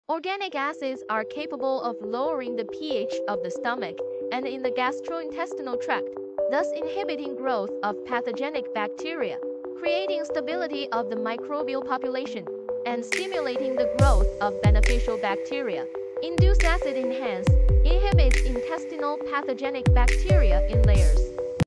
Antimicrobial property of acidifiers.
Organic acids are capable of lowering the pH of the stomach and in the gastrointestinal tract, thus inhibiting the growth of pathogenic bacteria, creating stability of the microbial population, and stimulating the growth of beneficial bacteria. InduceAcid Enhance, inhibits intestinal pathogenic bacteria in layers.